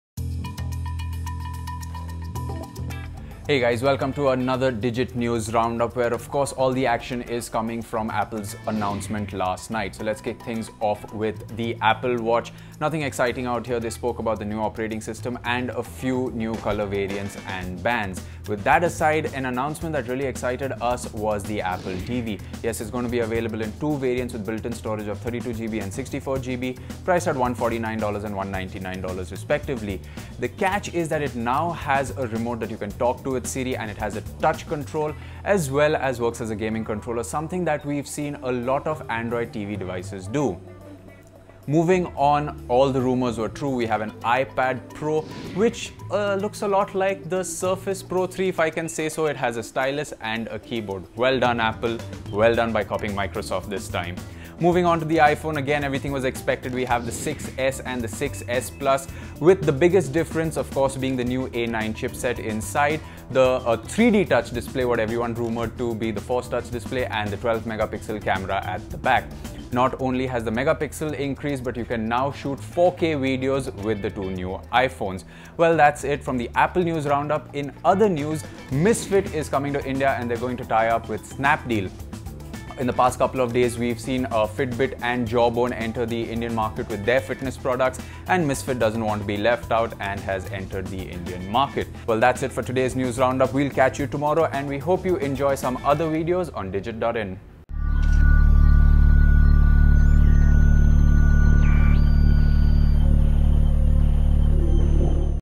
Digit News Headlines 10 Sep 2015 | Digit.in
Apple announces the iPad Pro, iPhone 6S and iPhone 6S Plus and an all new apple TV. In other news, Misfit is coming to India in an exclusive tie-up with Snapdeal.